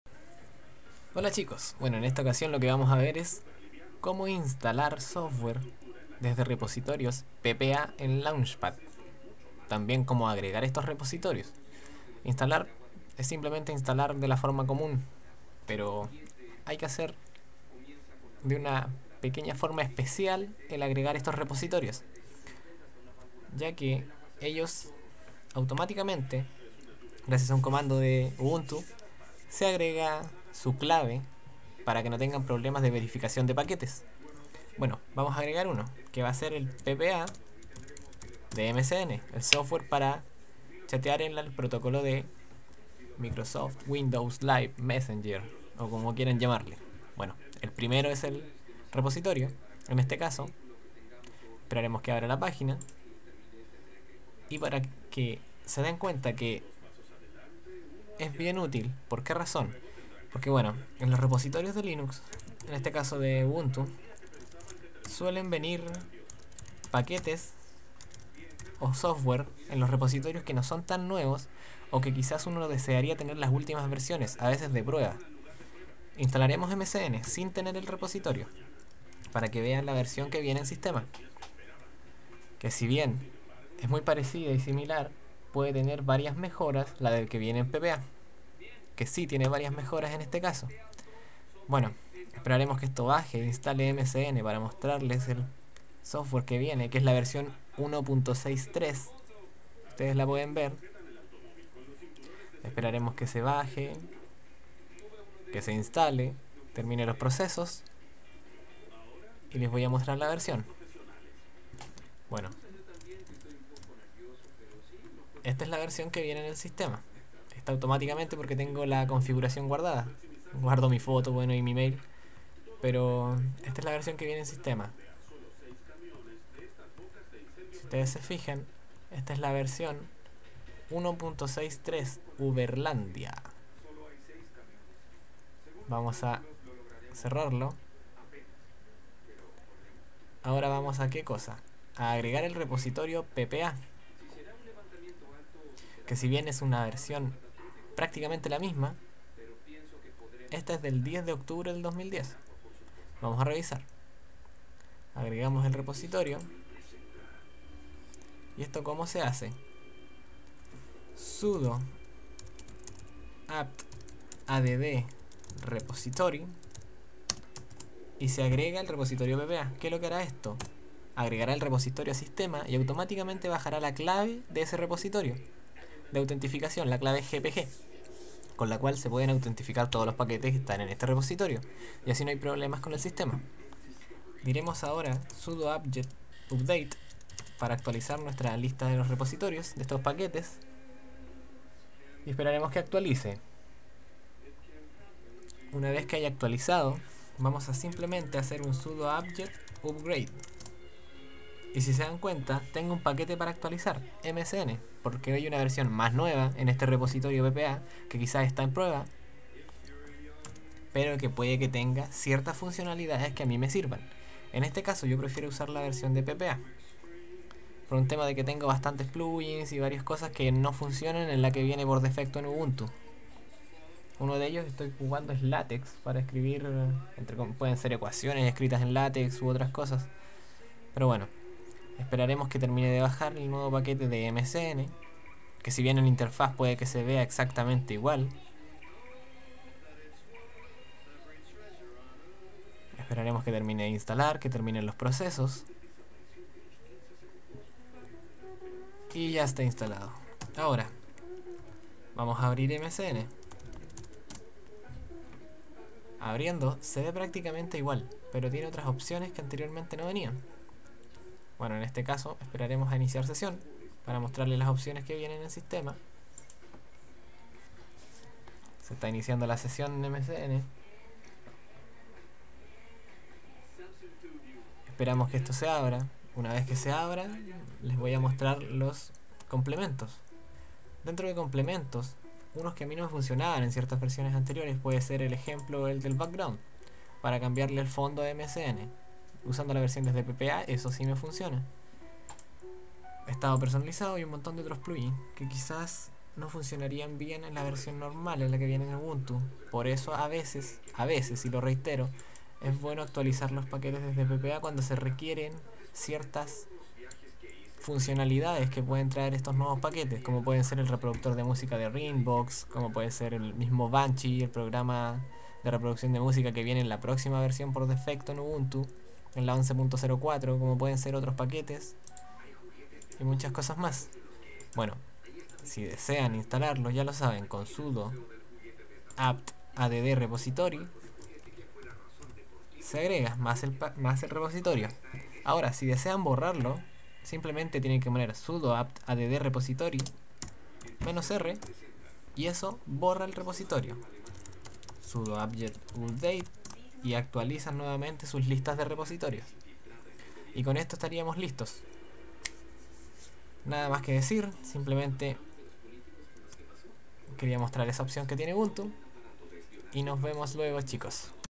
Agregar repositorios ppa de launchpad en ubuntu
Se muestra la forma de agregar repositorios ppa de launchpad en ubuntu, valido desde la version 9.10 de ubuntu en adelante.

Para realizar la muestra, se hizo con el repositorio ppa de emesene.